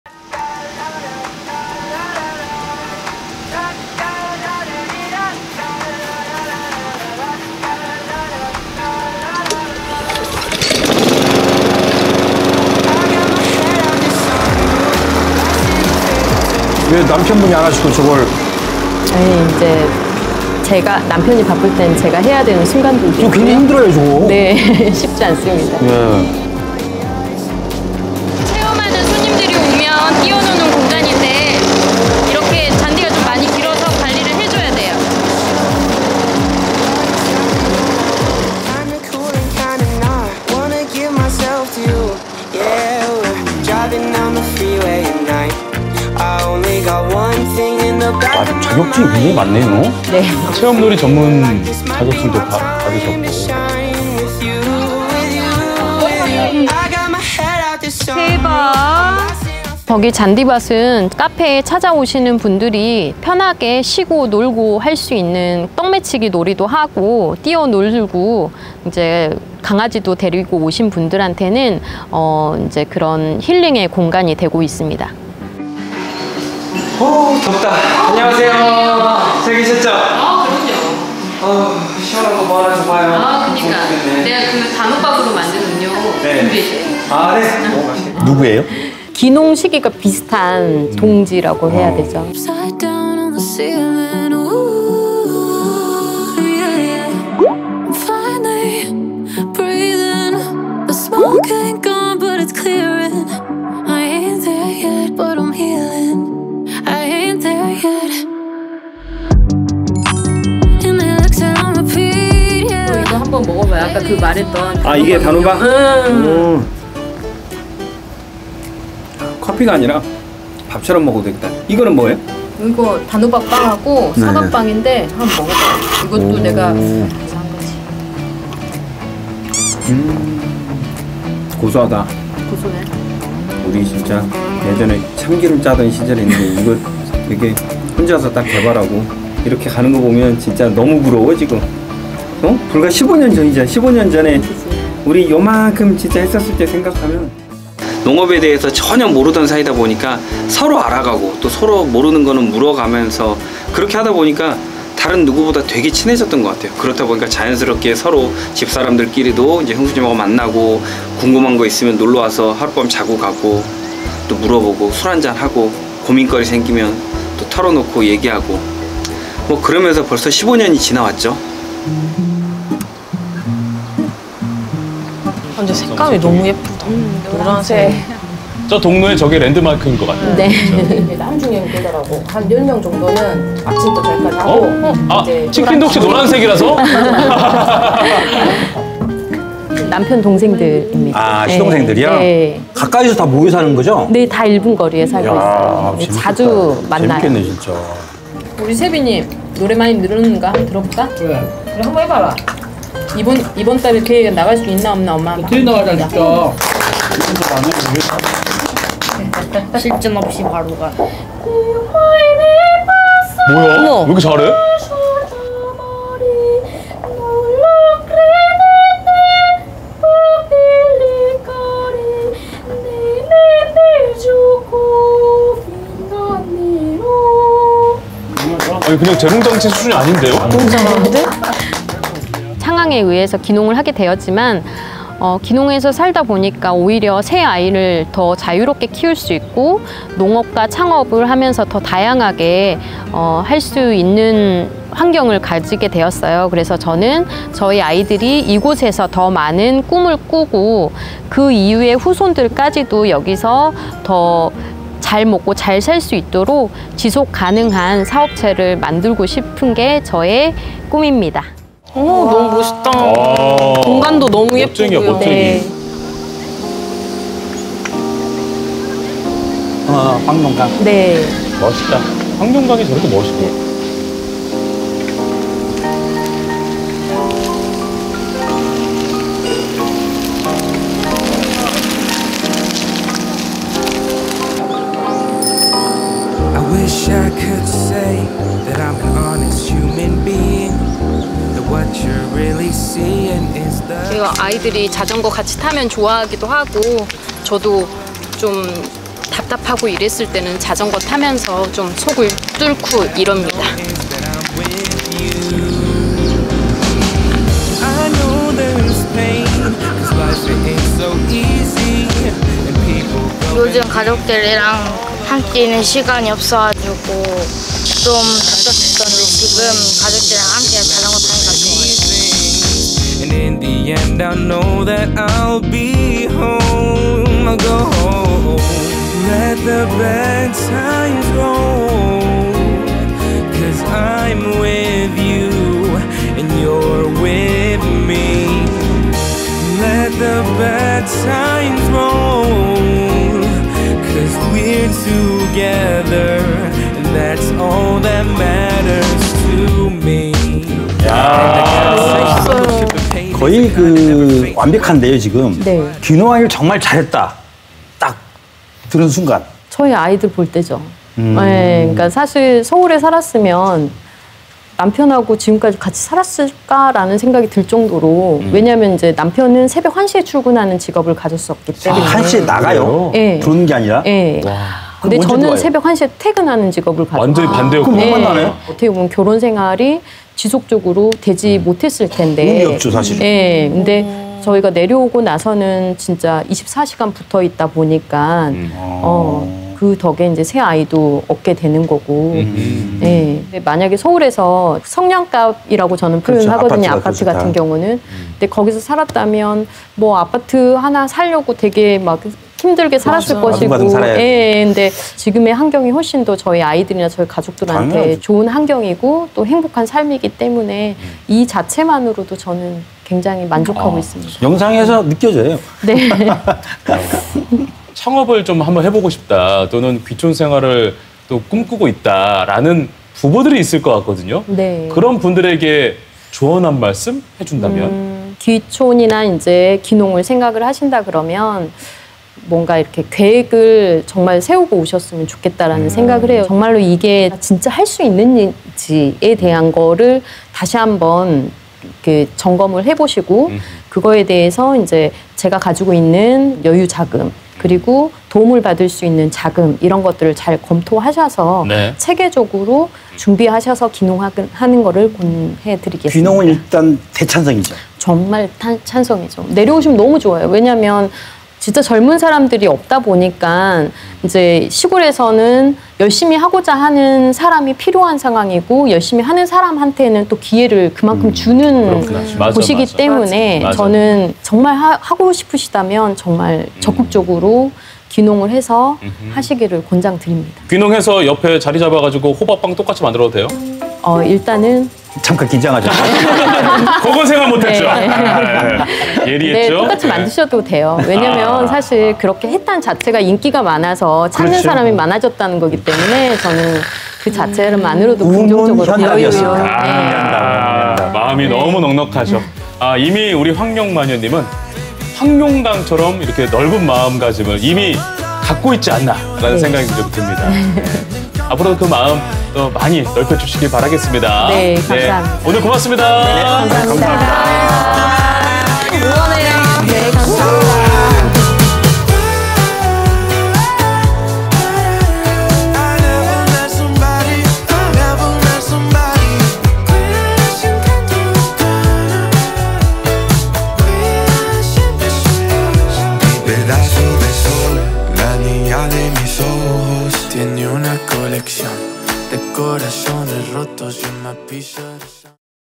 [마녀들의 포레스트] 도시를 벗어나 시골로 간 농부의 이야기｜귀촌｜귀농
도시를 벗어나 시골로 간 농부의 이야기